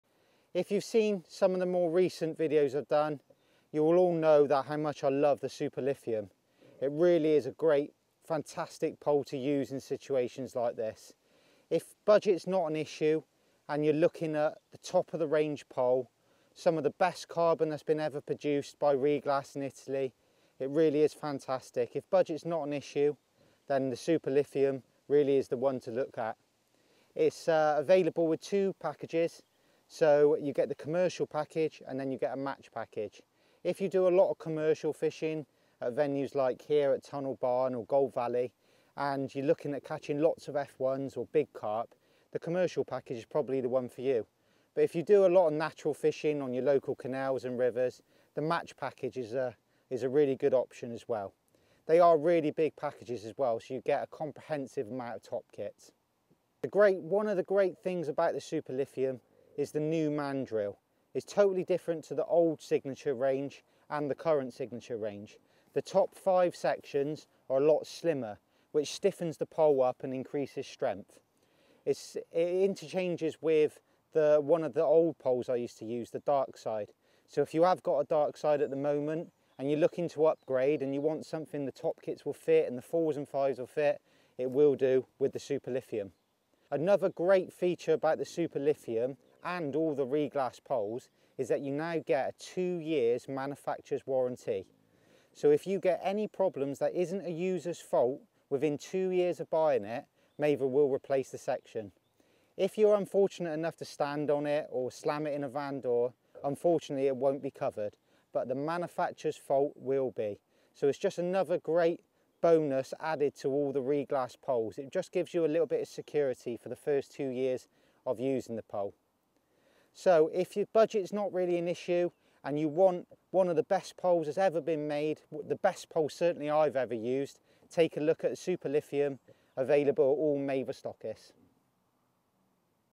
The Best Fishing Pole Ever. Maver Match Fishing TV:
A sensational new flagship pole from Maver for 2020. The Superlithium is possibly the greatest commercial pole we have ever produced, which is some statement considering our history of producing top-class poles. Manufactured from the highest level of super high-grade modulus carbon fibre, the Superlithium is finished in the elegant Suncore anti-friction surface utilising STER toughened epoxy resins and Nanolith technology. The top 5 will also interchange for those with the Darkside range of poles. The pole comes supplied with three different 2pc kits (all the same length), with the Matchlite kits constructed from the
highest grade of carbon available. The Powerlite kits have been modified to enhance their performance with a wider taper throughout to accommodate a 5mm bush. 2 bi-conical extensions are added to the package as well as classy pole end protectors. Designed and tested by Welsh International Lee Edwards, who has left no stone unturned in his quest to produce the strongest commercial pole we have ever seen. The flagship Superlithium is bursting with innovation, style and performance and is, without doubt, a real gem that we at Maver are incredibly proud of! The Superlithium comes supplied with a very comprehensive kit package and is available with a choice of two different kit options –
Match or Commercial.

Commercial Kit Package
1 x Powerlite kit inside the pole
4 x Power carp kits, slotted & bushed
5 x Powerlite kits, slotted & bushed
2 x 1pc Power kits
2 x Number 4 sections
2 x Short number 4
Pole end protectors on 11/13/16m sections
1 x Cupping kit & cups
1 x Bi-conical extension 6-7
1 x Bi-conical extension 8-9
Pole holdall & protective tubes
Match Kit Package
1 x Matchlite kit inside the pole
6 x Matchlite kits, slotted & bushed
3 x Powerlite kits, slotted & bushed
3 x Number 4 sections
1 x Short number 4
1 x Cupping kit & cups
1 x Bi-conical extension 6-7
1 x Bi-conical extension 8-9